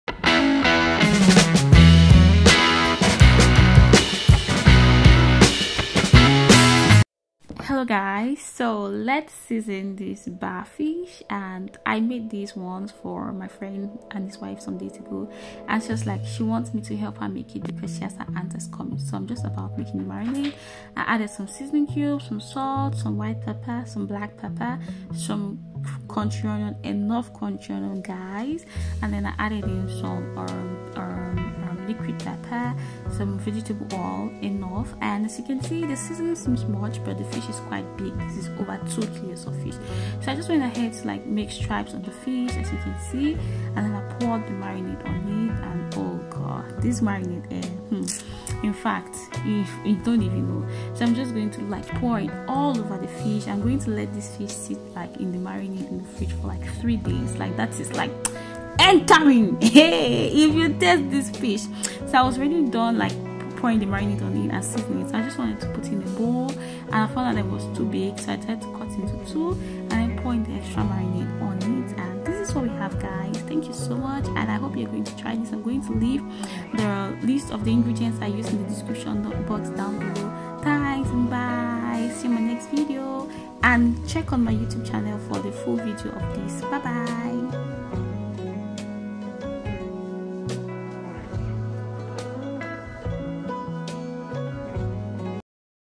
LET'S MARINATE THE MOST DELICIOUS GRILLED FISH YOU WILL EVER EAT!!!!
Hello guys on today's video we will be marinating fish. you can either fry this or bake it in the oven.

Salut les gars aujourd'hui on va mariner le poisson 😋😋😋.

ingredients:
fish
salt
pepper
white and black pepper
seasoning cube
country onion
vegetable oil

ingrédients:
sel
cube
poisson
poivre blanc et noir
piment
Rondelles